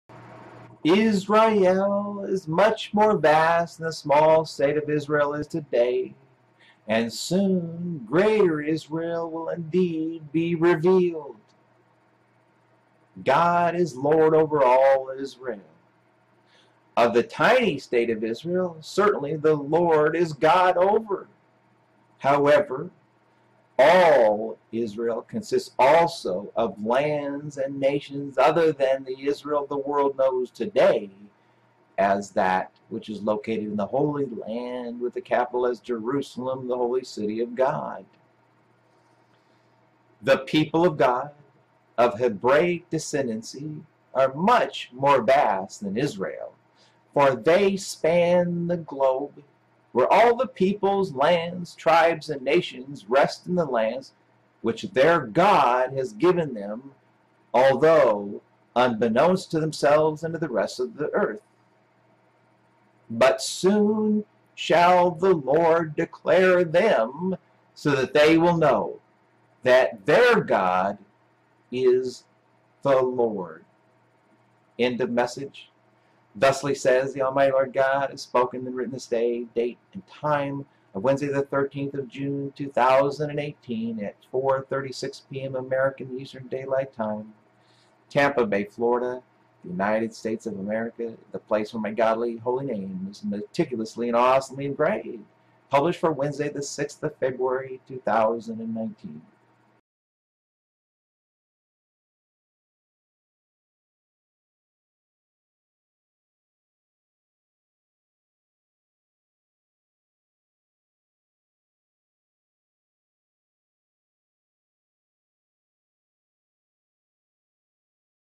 Israel is More Vast than the Tiny State of Israel is Today, & Soon, Greater Israel will be Revealed!
Israel is Much More Vast than the Small State of Israel is Today … and Soon, Greater Israel will Indeed be Revealed …

Published … for Wednesday, the 6th of February 2019.

483.32---b.01